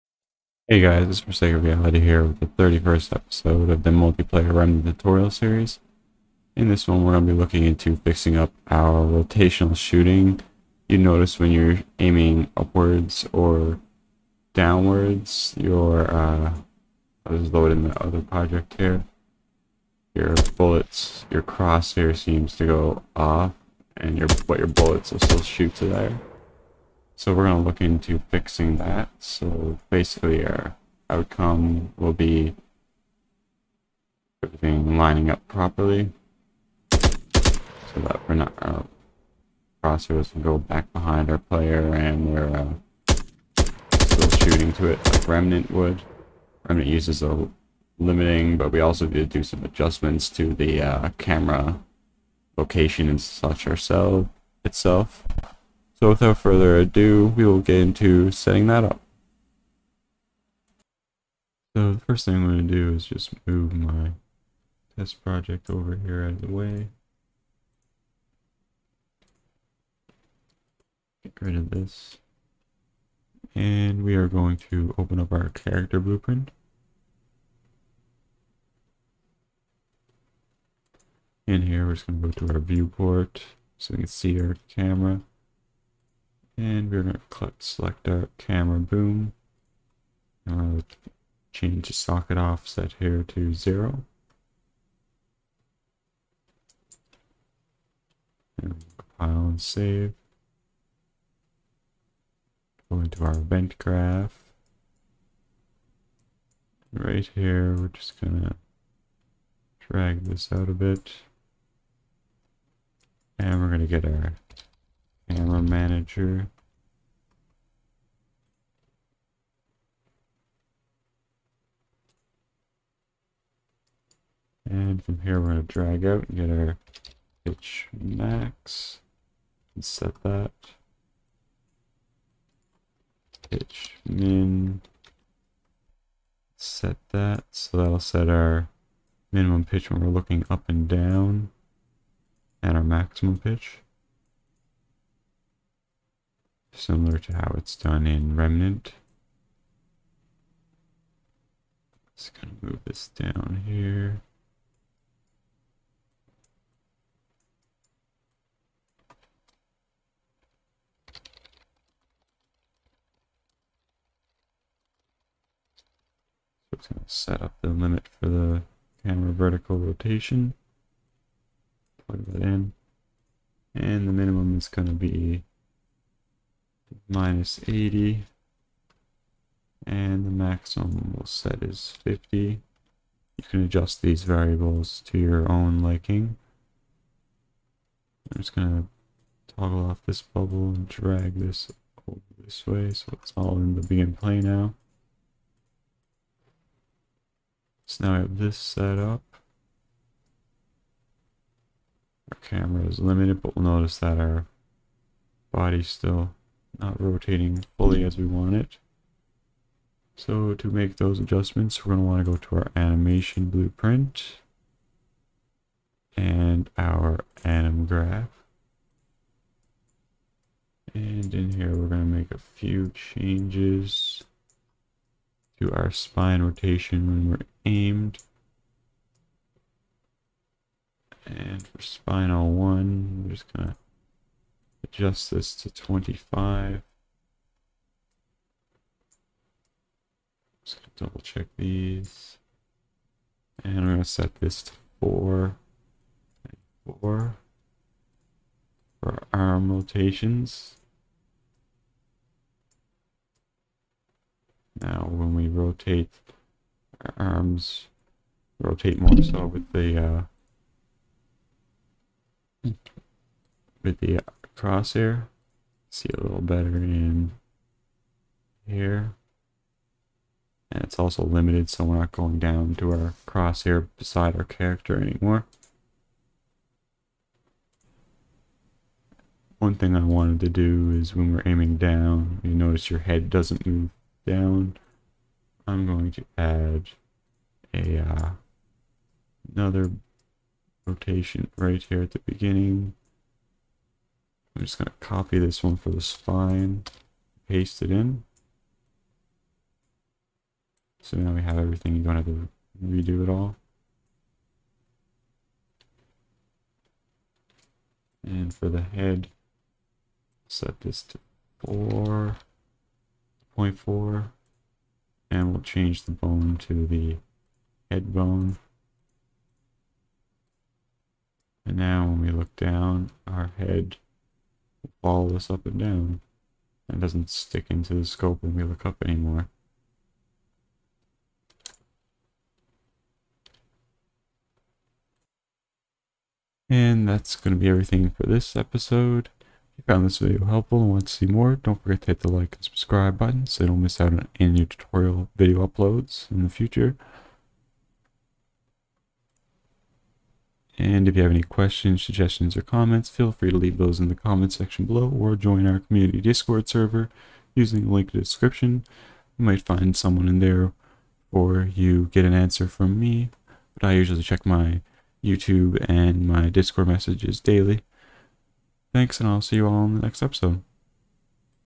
Crosshair Offset Fixes - #31: How To Make a Third Person Shooter Like Remnant in Unreal Engine 4
Crosshair offset and Better Spine Rotation - #31: How To Make a Third Person Shooter Like Remnant in Unreal Engine 4

Remnant is a Multiplayer Co-op Third Person Shooter Game that you can play with up to 4 people (3 other friends) at once. in this episode we will make some adjustments to our camera to allow the Crosshair to be in the correct location and limit our camera rotation to prevent rotation issues. We will also set up a better looking spine rotation when aiming that will adjust the head bone as well.